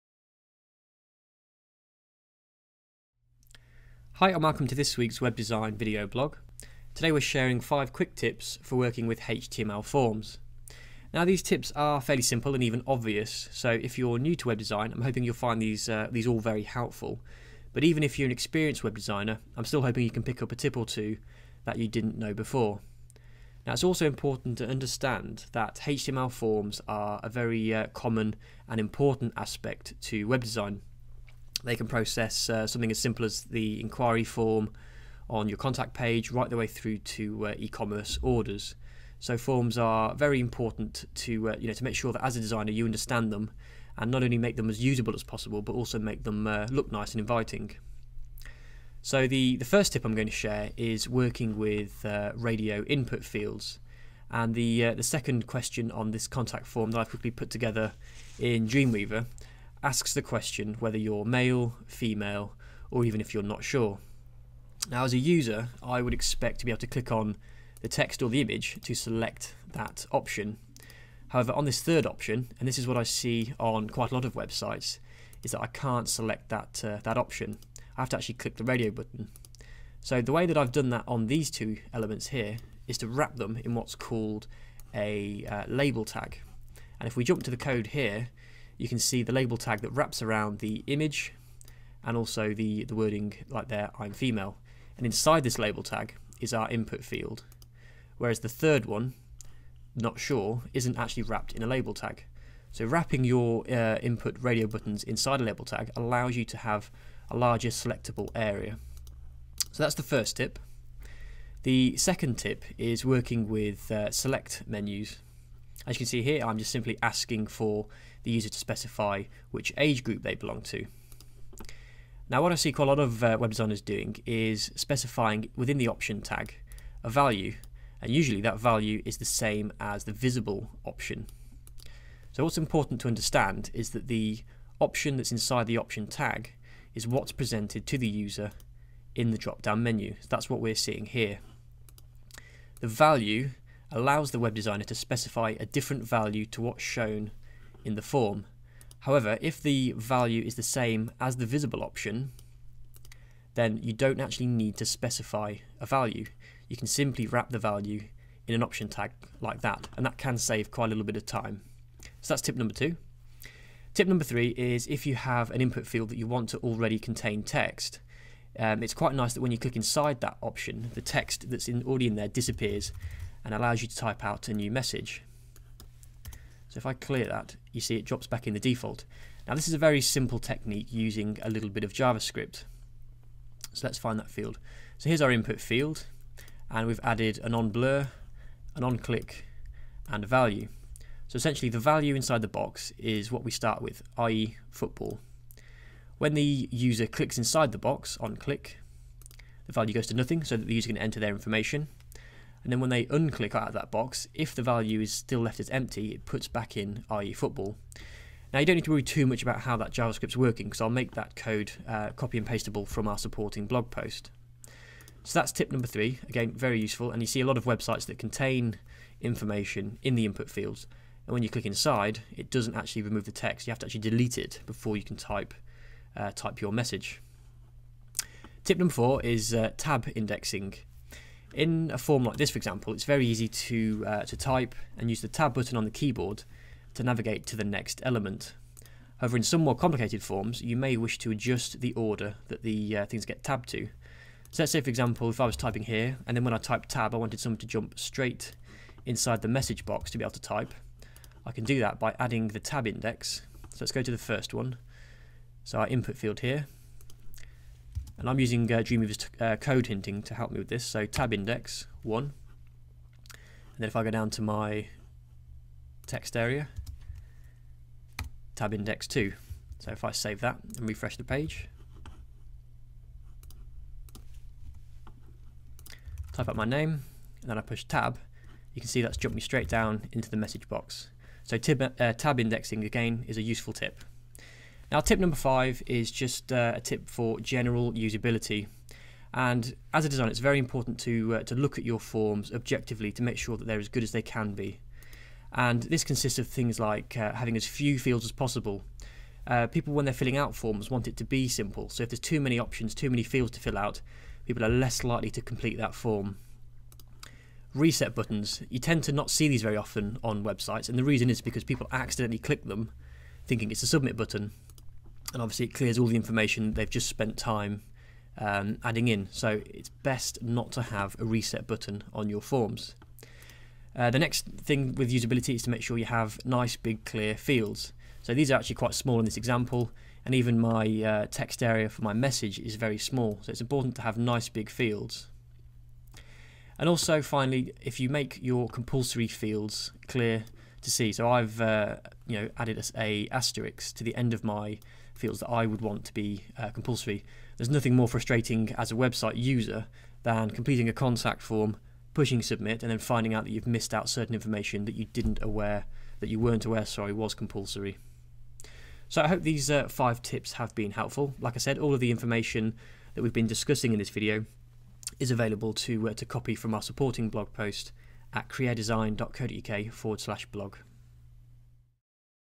5 Quick Tips for HTML Forms
In this week's web design video, I share 5 quick and simple tips for creating forms in HTML. Tips include Tab Indexing, Drop-Down Options and using JavaScript to increase usability. Find the code on our supporting blog post